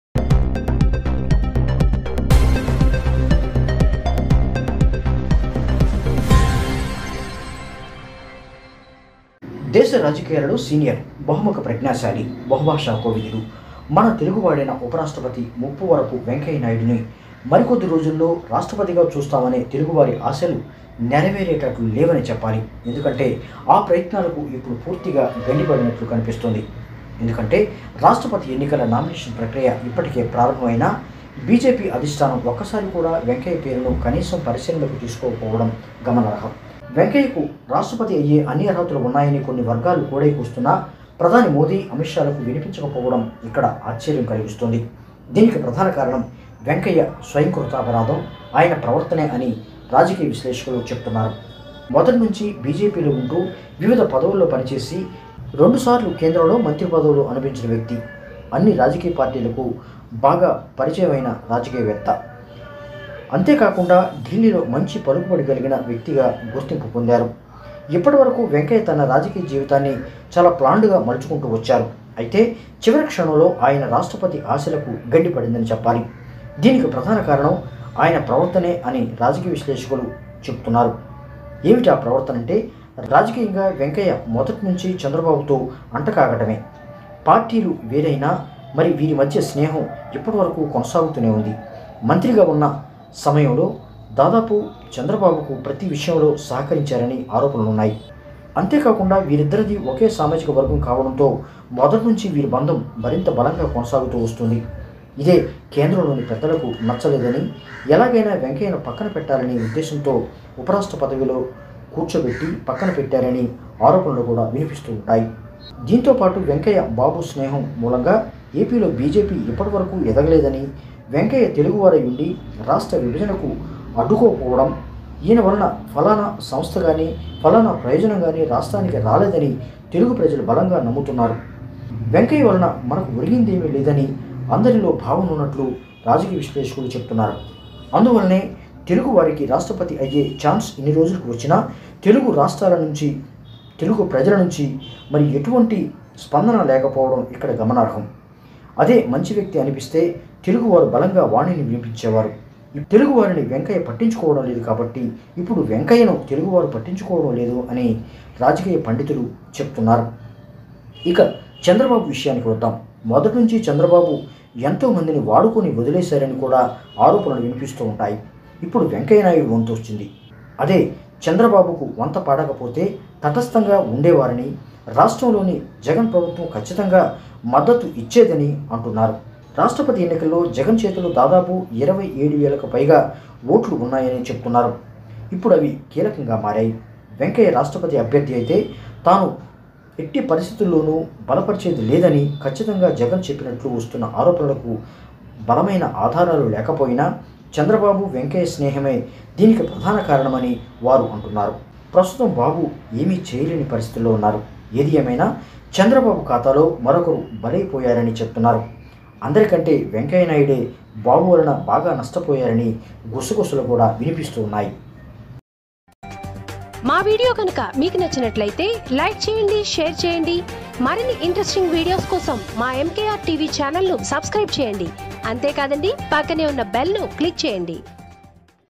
రాష్ట్రపతి కాకుండా వెంకయ్యను అడ్డుకున్నది ఎవరు? || MKR tv
రాష్ట్రపతి కాకుండా వెంకయ్యను అడ్డుకుంటుంది ఎవరు? || MKR tv